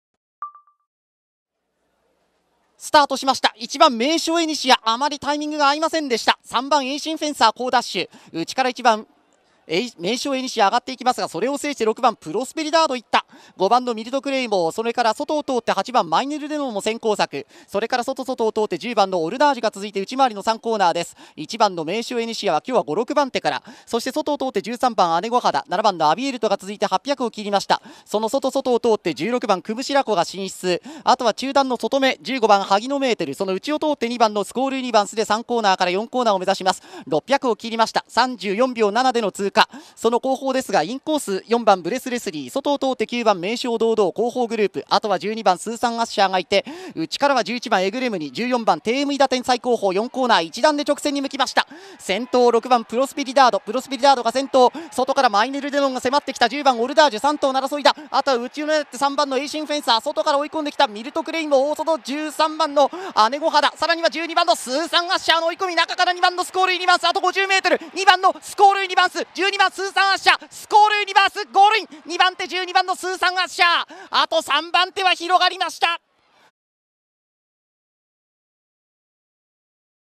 Squall Yunibansu (JPN) wins race 12 at Hanshin 12/28/2028
12/28/2023 Hanshin Race 12 Countdown Stakes 1200m (Turf)
Squall Yunibansu (pgm#2)

Squall Yunibansu (JPN)
TB, B, M, Foaled January 8, 2019
Duramente (JPN) - Caledonia Lady (GB), by Firebreak (GB)
Breeder: Northern Farm